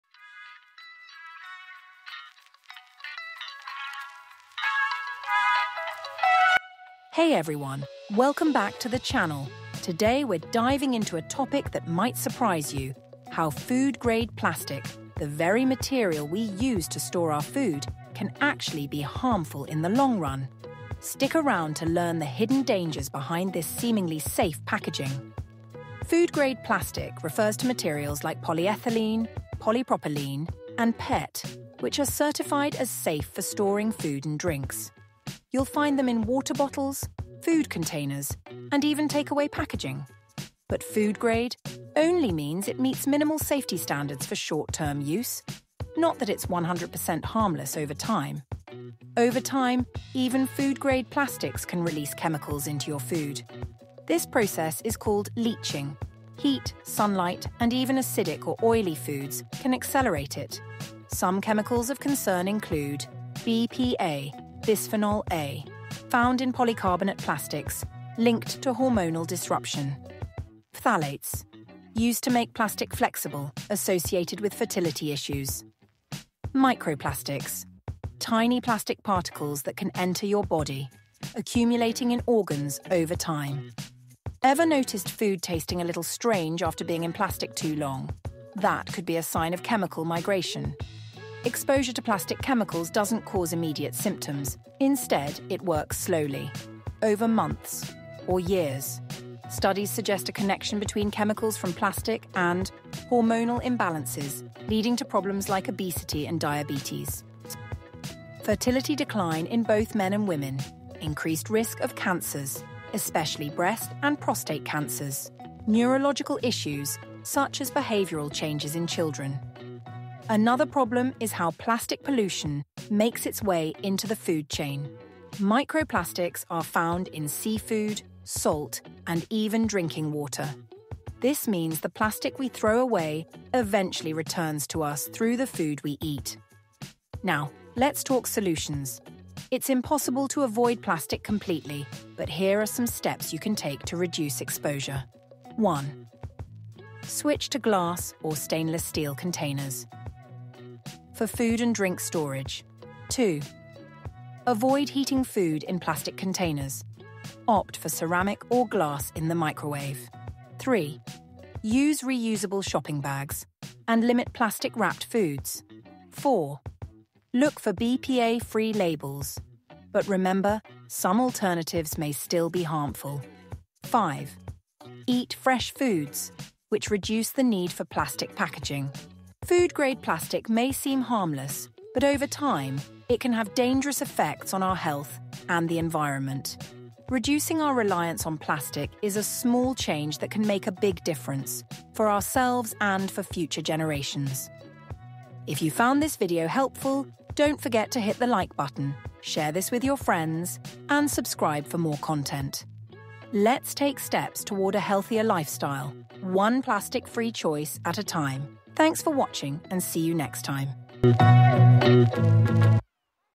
Food Grade Plastic Kills
"HOW FOOD-GRADE PLASTIC KILLS SLOWLY"

**BPA (Bisphenol A):** Found in polycarbonate plastics, linked to hormonal disruption.
- **Phthalates:** Used to make plastic flexible, associated with fertility issues.
- **Microplastics:** Tiny plastic particles that can enter your body, accumulating in organs over time.

"Exposure to plastic chemicals doesn’t cause immediate symptoms. Instead, it works slowly—over months or years. Studies suggest a connection between chemicals from plastic and:"

- Hormonal imbalances, leading to problems like obesity and diabetes.
- Fertility decline in both men and women.
- Increased risk of cancers, especially breast and prostate cancers.
- Neurological issues, such as behavioral changes in children.

1. **Switch to glass or stainless steel containers** for food and drink storage.
2. **Avoid heating food in plastic containers**—opt for ceramic or glass in the microwave.
3. **Use reusable shopping bags** and limit plastic-wrapped foods.
4. **Look for BPA-free labels,** but remember, some alternatives may still be harmful.
5. **Eat fresh foods,** which reduce the need for plastic packaging

Let’s take steps toward a healthier lifestyle, one plastic-free choice at a time.